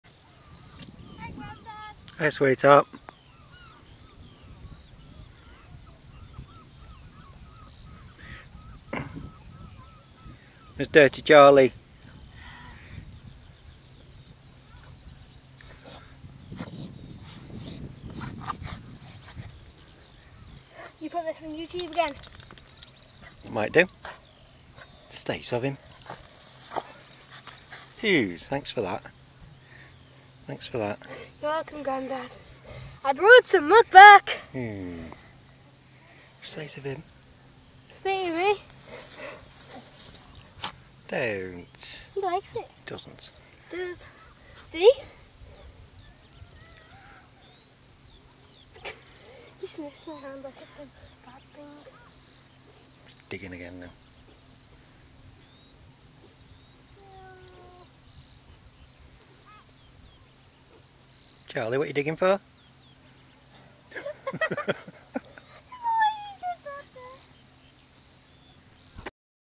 Charlie comes back muddy
Charlie and Hannah come back from the sea a bit mucky.